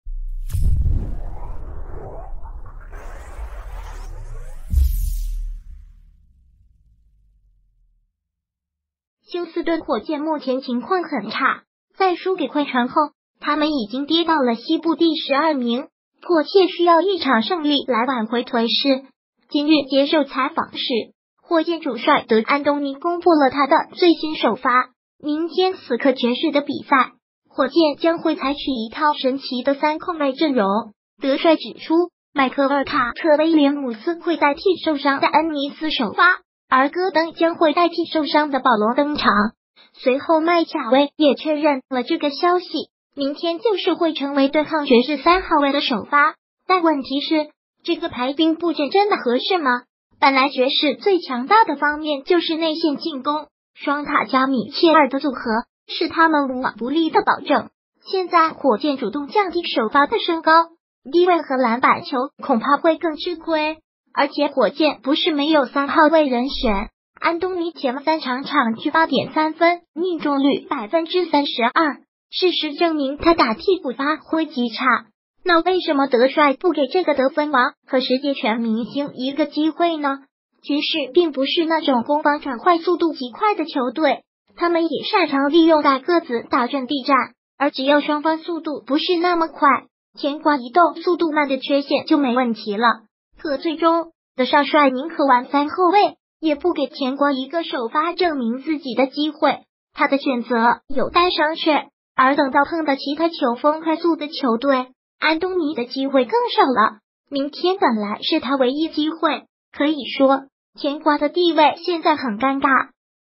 火箭提前曝光首发，德帅拒绝给甜瓜最后机会，3后卫神阵死磕双塔
休斯顿火箭目前情况很差，在输给快船后，他们已经跌到了西部第12名，迫切需要一场胜利来挽回颓势。今日接受采访时，火箭主帅德安东尼公布了他的最新首发，明天死磕爵士的比赛，火箭将会采取一套神奇的三控卫阵容。德帅指出，迈克尔-卡特-威廉姆斯会代替受伤的恩尼斯首发，而戈登将会代替受伤的保罗登场。随后迈卡威也确认了这个消息，明天就是会成为对抗爵士3号位的首发，但问题是，这个排兵布阵真的合适吗。本来爵士最强大的方面就是内线进攻，双塔+米切尔的组合是他们无往不利的保证，现在火箭主动降低首发的身高，低位和篮板球恐怕会更吃亏。而且火箭不是没有三号位人选。安东尼前三场场均8.3分命中率32%，事实证明他打替补发挥极差，那为什么德帅不给这个得分王和10届全明星一个机会呢？爵士并不是那种攻防转换速度极快的球队，他们也擅长利用大个子打阵地战，而只要双方速度不是那么快，甜瓜移动速度慢的缺陷就没问题了。可最终，德帅宁可玩三后卫，也不给甜瓜一个首发证明自己的机会，他的选择有待商榷。而等到碰到其他球风快速的球队，安东尼的机会更少了，明天本来是他唯一机会。可以说，甜瓜的地位现在很尴尬。